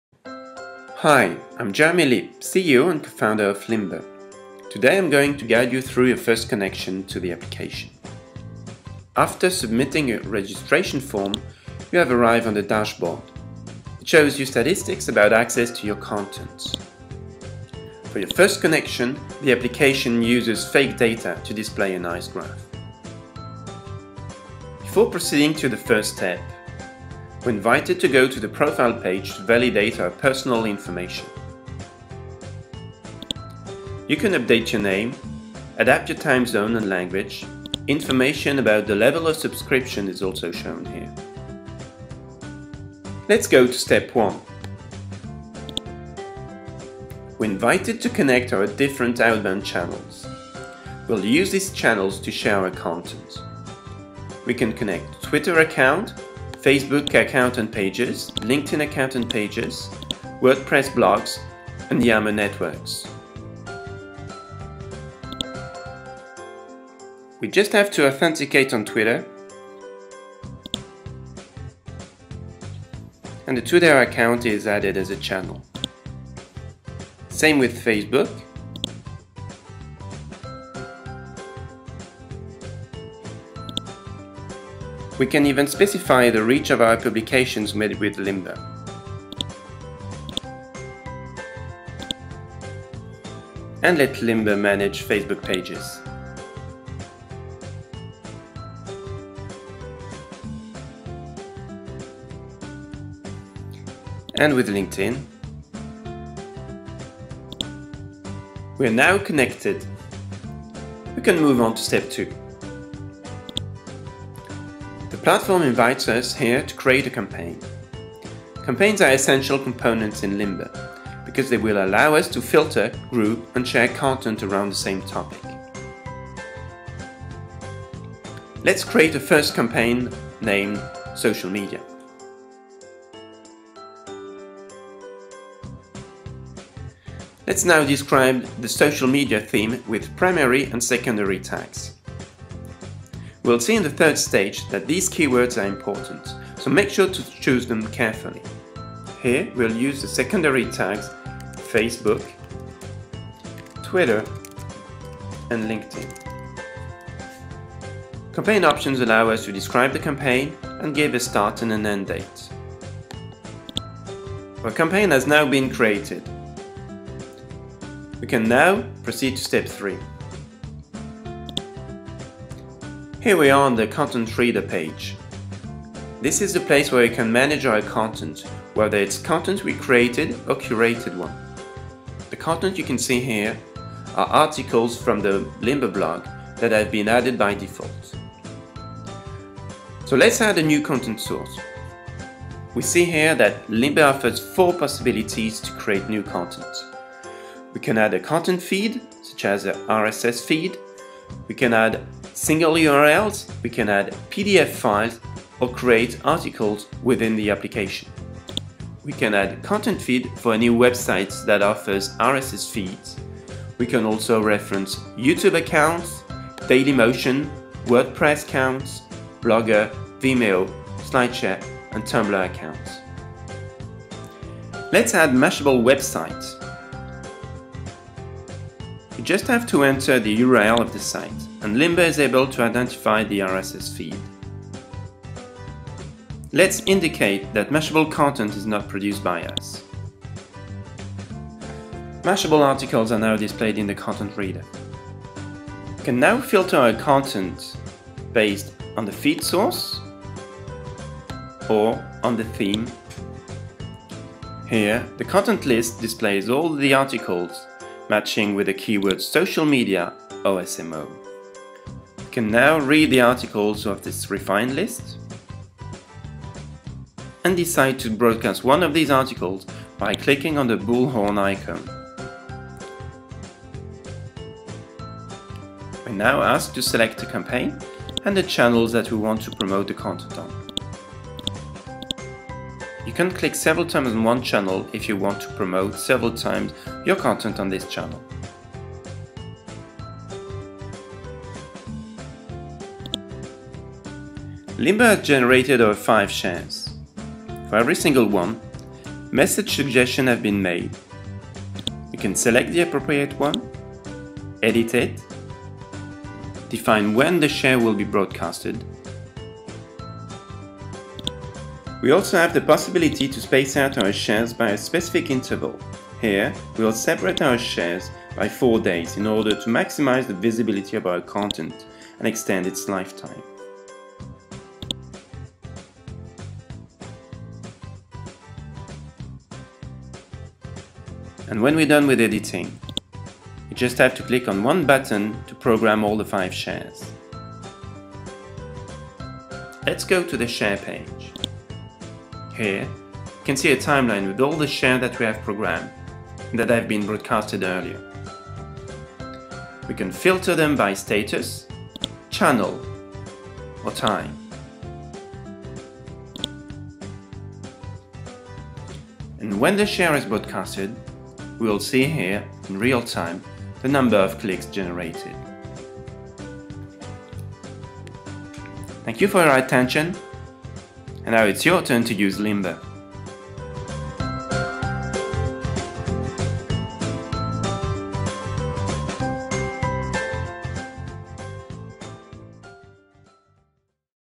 Guided Tour: Your first connection to Limber
This videos guides you through the Content & Social Marketing platform Limber and helps you configure it.